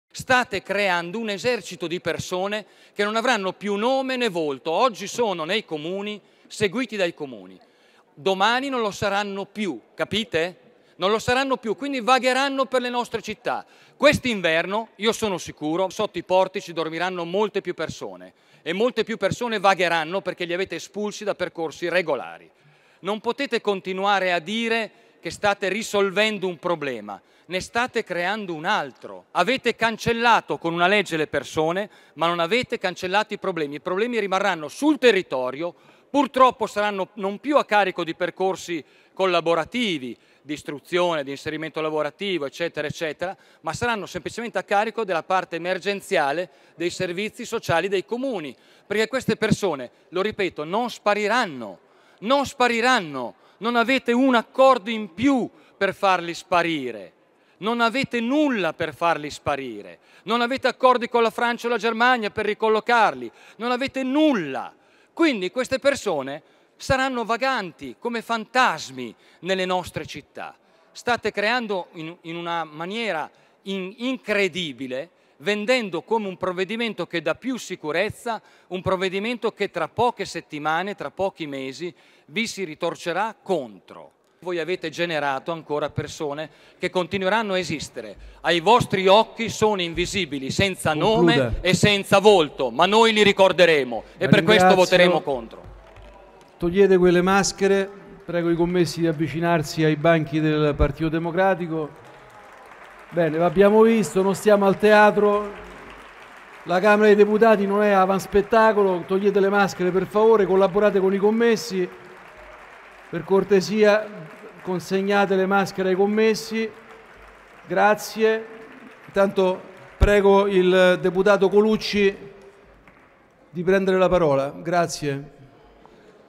Decreto sicurezza, Pd protesta con le maschere in aula: "Governo crea un esercito di irregolari"
"State creando centomila persone senza più nome né volto, oggi sono seguite dai comuni, domani saranno espulse dai percorsi regolari ma non spariranno: vagheranno come fantasmi per le nostre città". Così il capogruppo Pd alla Camera Graziano Delrio nella dichiarazione di voto sul decreto sicurezza, criticando la stretta del governo sul circuito degli Sprar e sui permessi umanitari per i richiedenti asilo. I deputati Pd hanno protestato indossando delle maschere bianche a simboleggiare quello che Delrio ha definito "l'esercito di nuovi migranti irregolari" che secondo i dem il decreto è destinato a generare. .  .di Marco Billeci